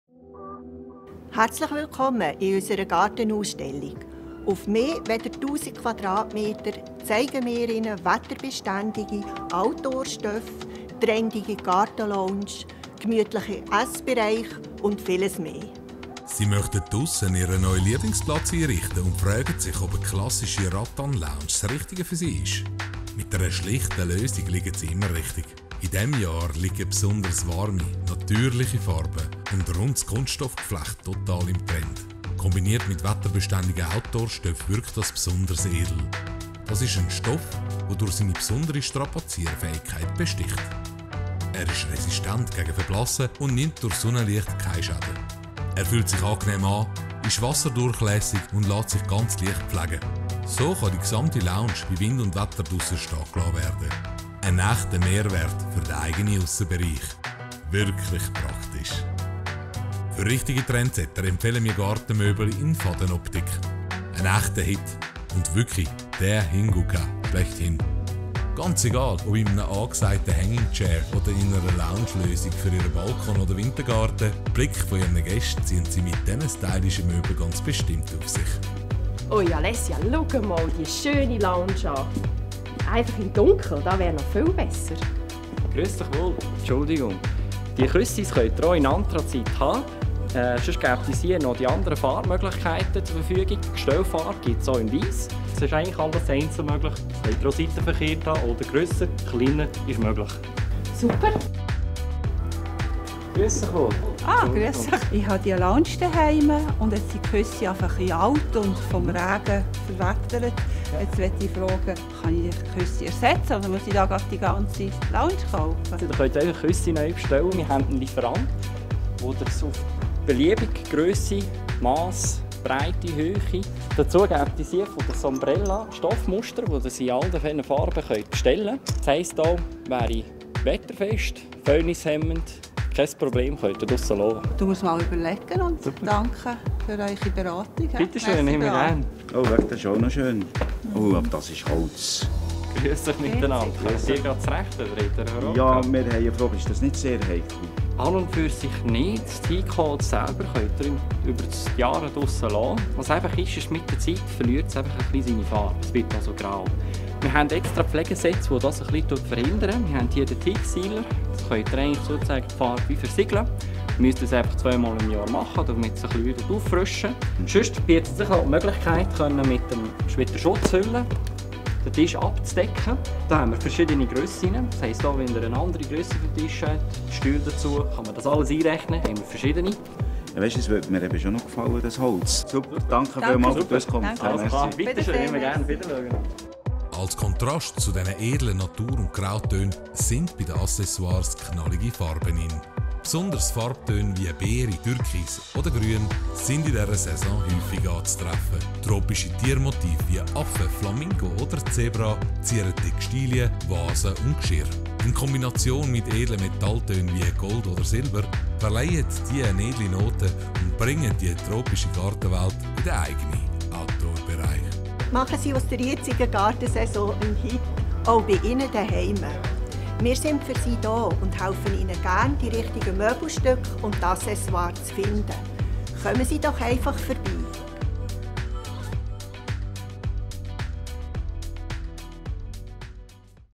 Gartentrends 2019
Auf über 1000m2 präsentiert Möbel Hubacher das saisonale Gartensortiment 2019. Hochwertige Möbel aus Teak, Edelstahl, Aluminium oder Geflecht zieren die grossflächige Ausstellungsfläche. Bei Möbel Hubacher stehen dieses Jahr besonders individuelle Bedürfnisse der Kunden im Fokus. Lounges, Tische und Accessoires in unterschiedlichen Farben und Materialien, lassen sich nach Belieben zusammenstellen. So hat jeder Kunde die Möglichkeit, aus dem normalen Gartenangebot sein eigenes, ganz persönliches Modell zu designen.